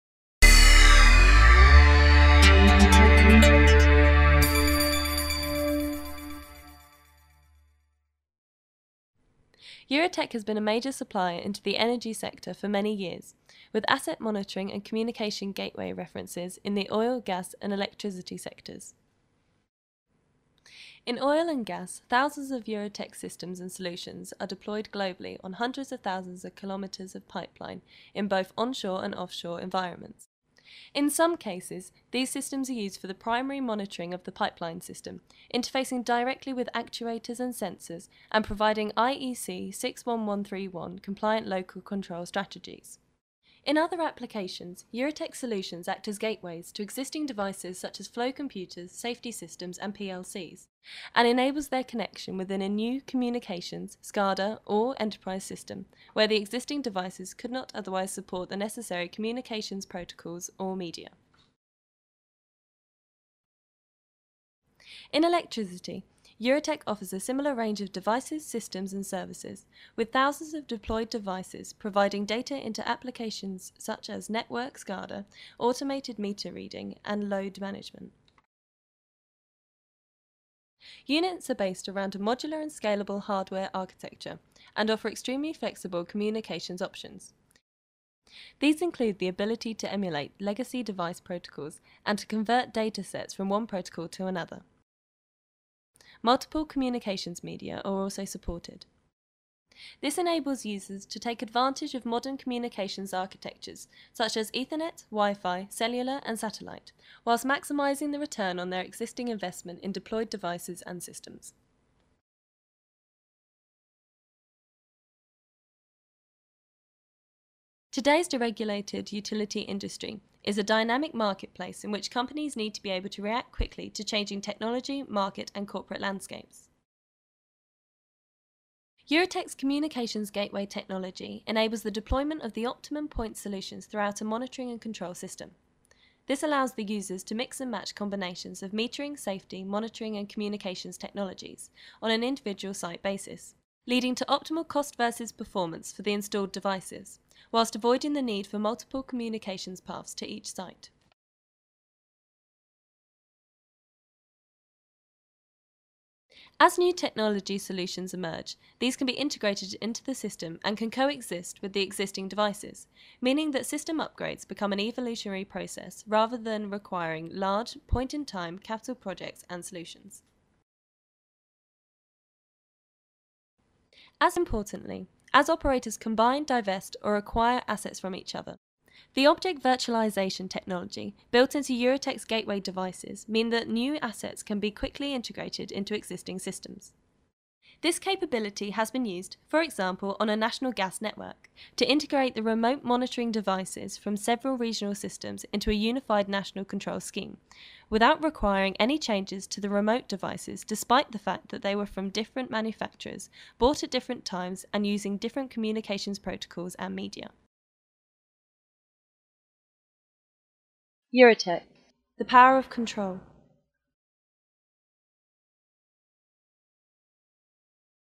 Eurotech Energy Solutions
Eurotech has been since many years a major supplier of the energy, oil and gas, power and electricity industry sectors.
Products are used in pipeline monitoring, smart meters automatic data collection and load management, SCADA (Supervisory Control And Data Acquisition) applications. Eurotech supplies also communication protocols converters and gateways for wired and wireless networks.